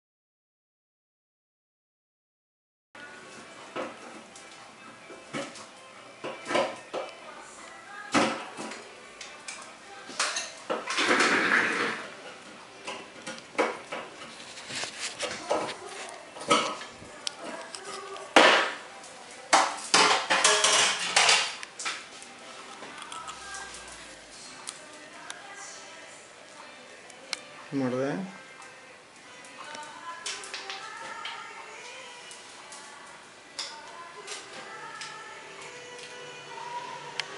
Estetica Dental.Alta Odontologia Estetica.
Odontologia estetica. Impresionante mimetizacion de porcelanas. Dr Lombardi Maximiliano. Odontologia Ortodoncia implantes